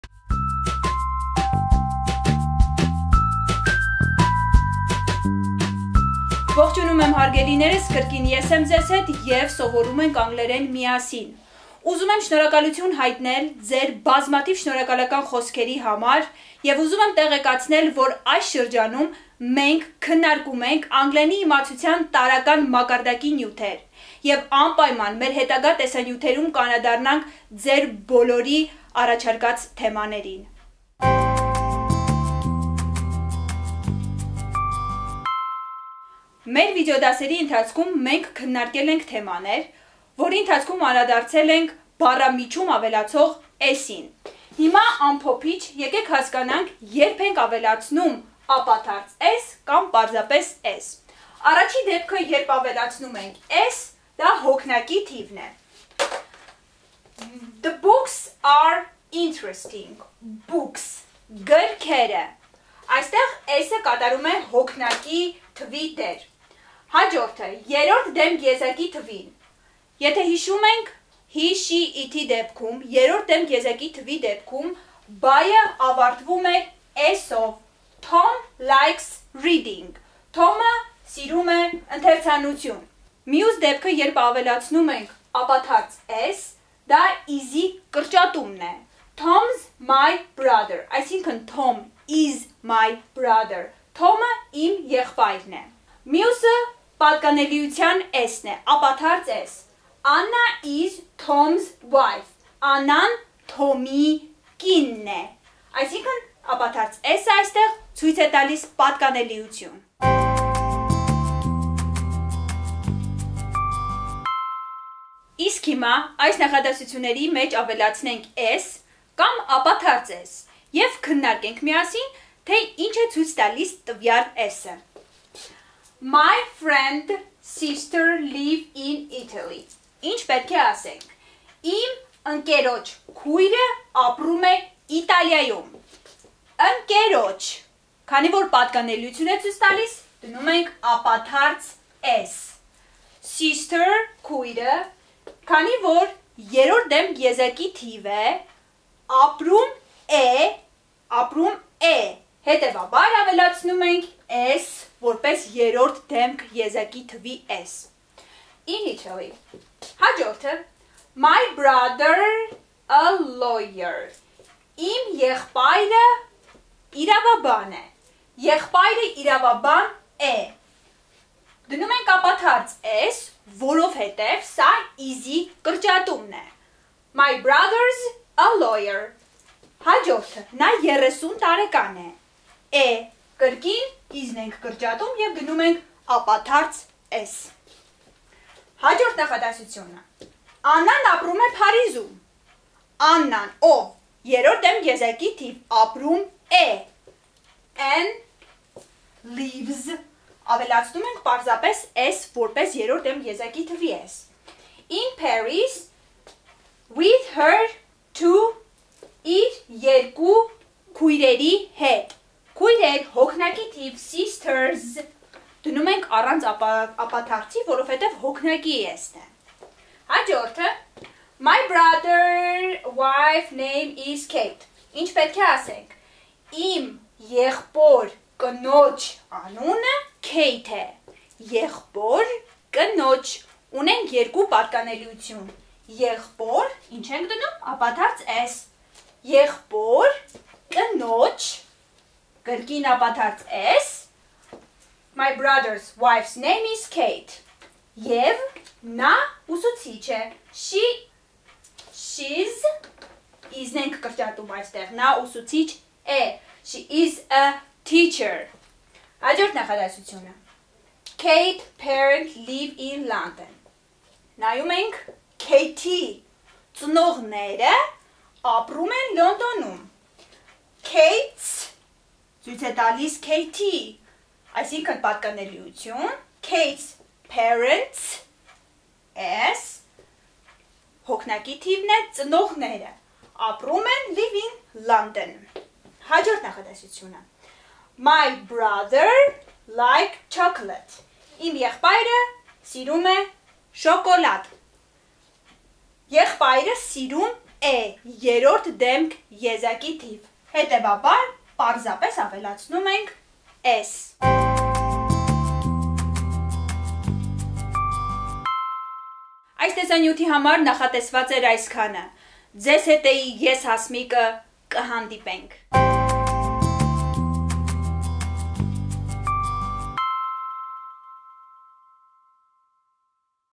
Դաս 18 / անգլերեն ('s / s)
S վերջավորության կիրառումը անգլերենում ( 's / s )

կարող եք հետևել մեզ նաև Facebook ում